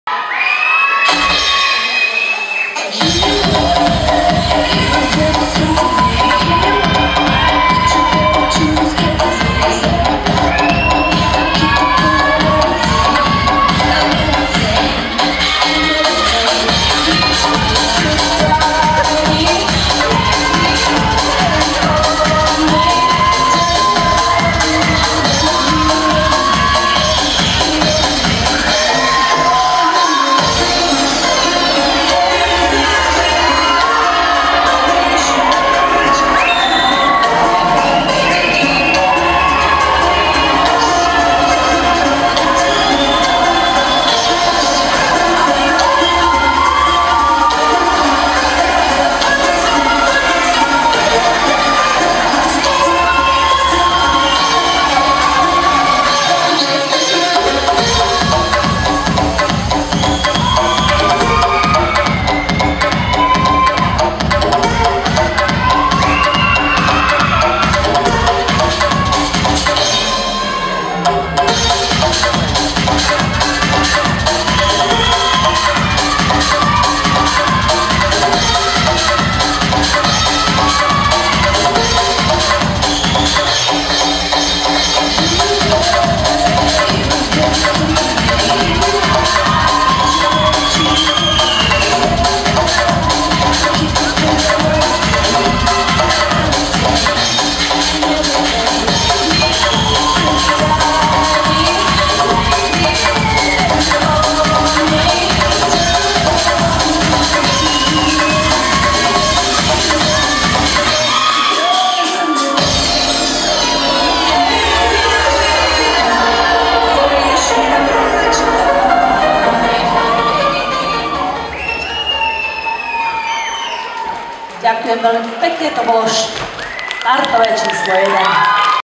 GRIMMY Prešov - Disco Dance Skupina HVK 2015
Tanečný pohár a Majstrovstvá SR v tancoch IDO 2015